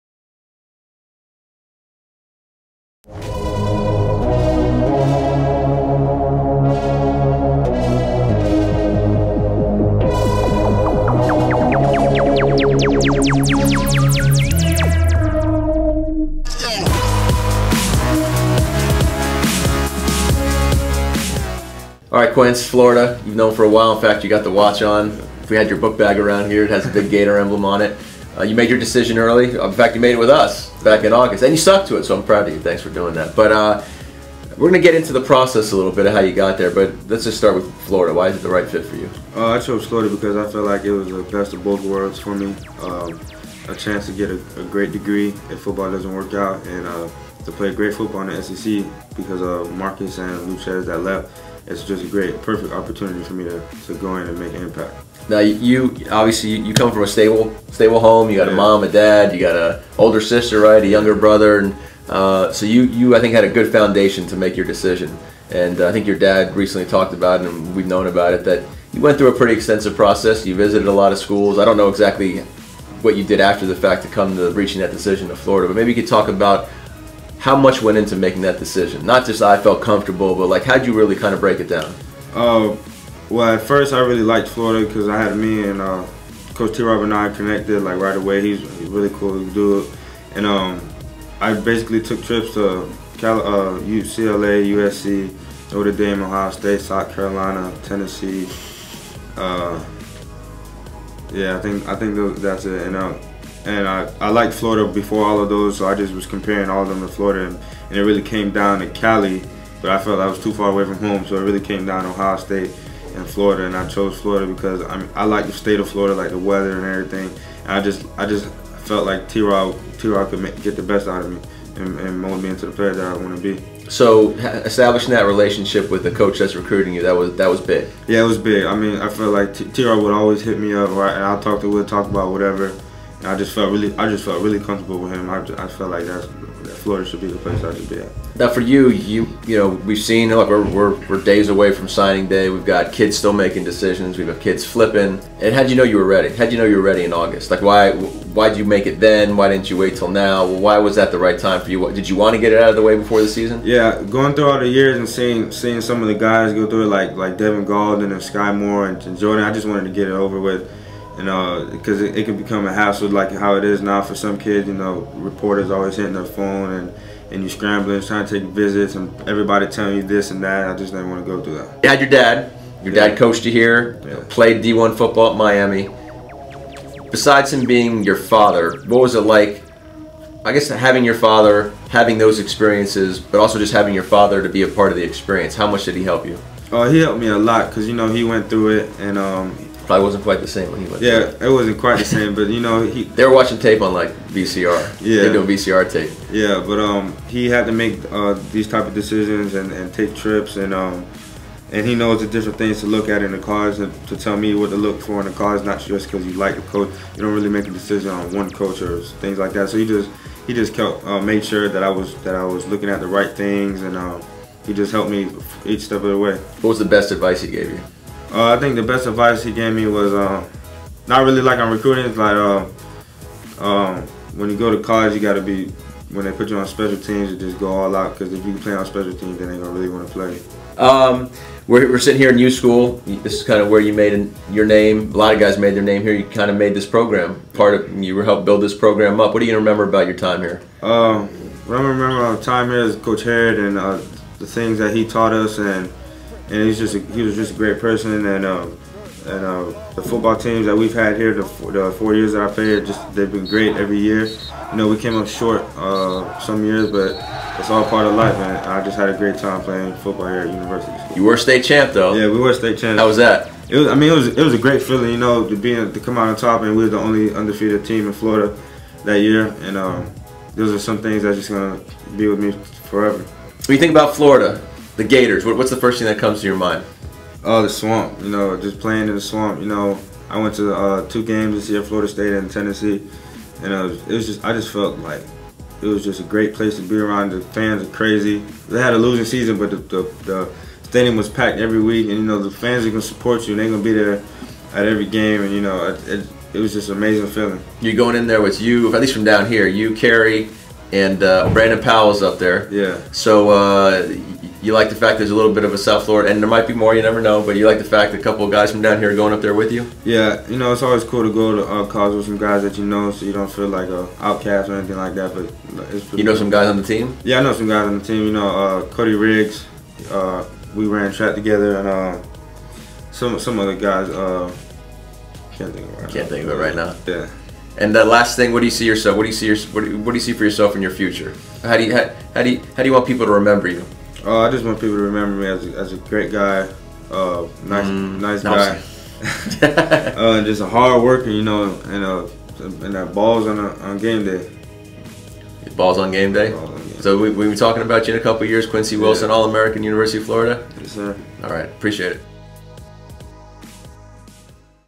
Signing day Spot Light Quincy Wilson
Josh Darrow sits down with future Florida Gator Quincy Wilson.